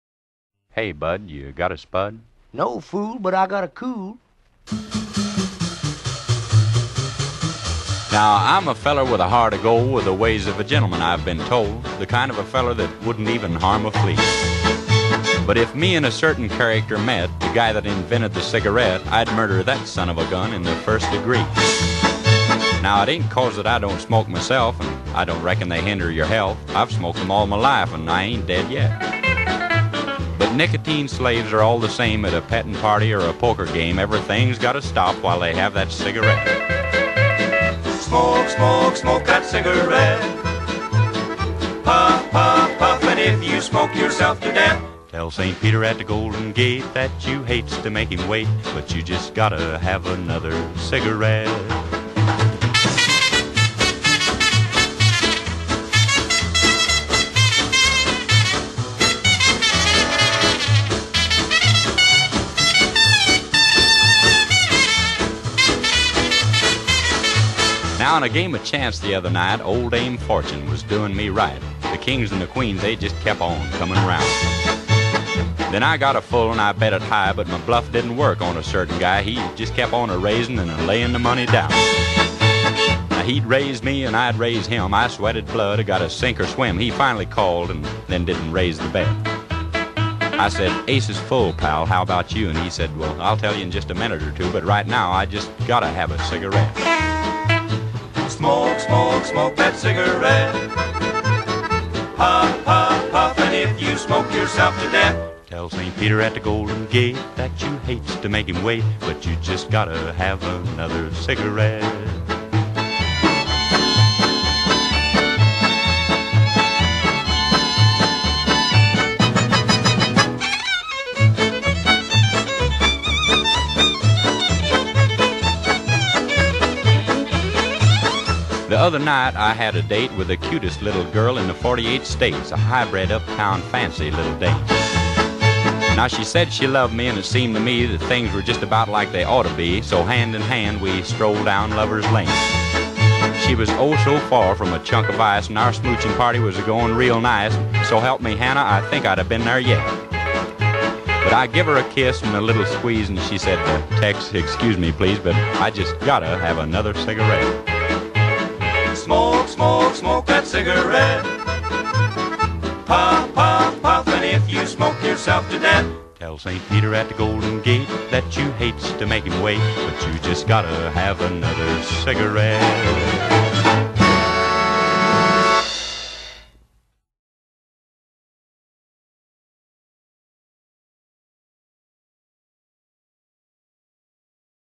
smoke somke that cig
ans if you need help go to smokershelpline.ca .and no mom i dont smoke it just a massage from everyone at miss j and the am and jenffer's show and me ok mom.